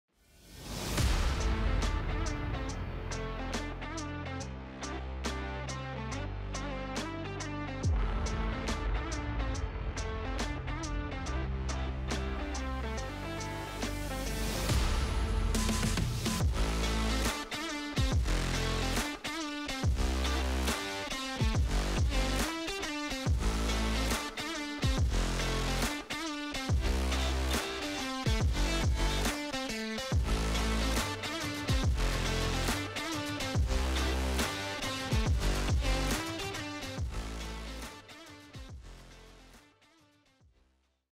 Photo Session Backstage Sneak Peak!
We had a absolute blast during photo session with our employees!

Get ready as we unveil some behind-the-scenes footage from one of our electrifying campaign sessions with our awesome employees.